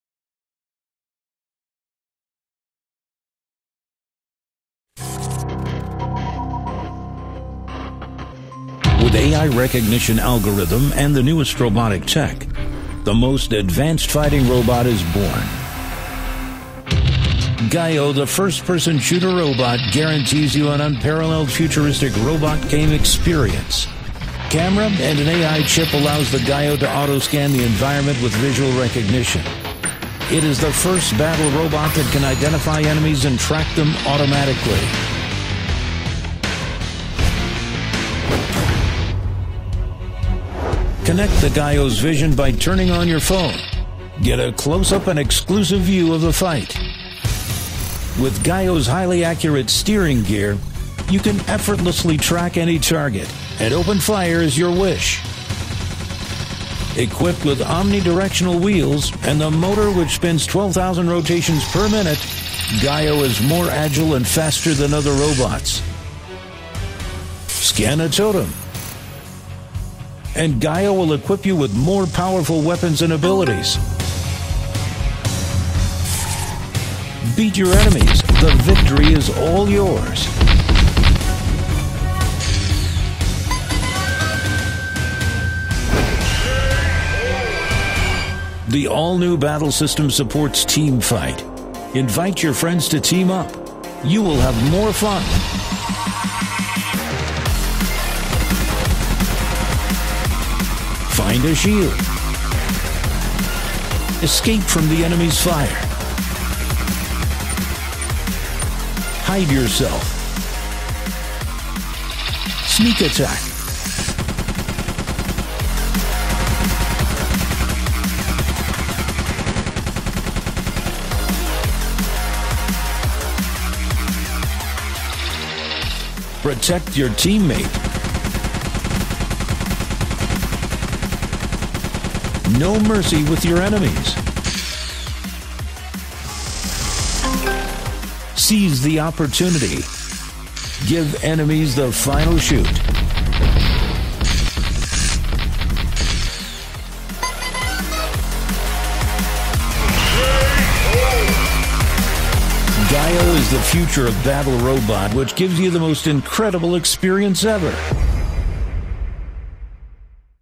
GEIO: A FPS Battle Bot With Visual Recognition
Battle with your friends: GEIO’s embedding AI and FPS gives you an immersive experience in a virtual war zone that exists in reality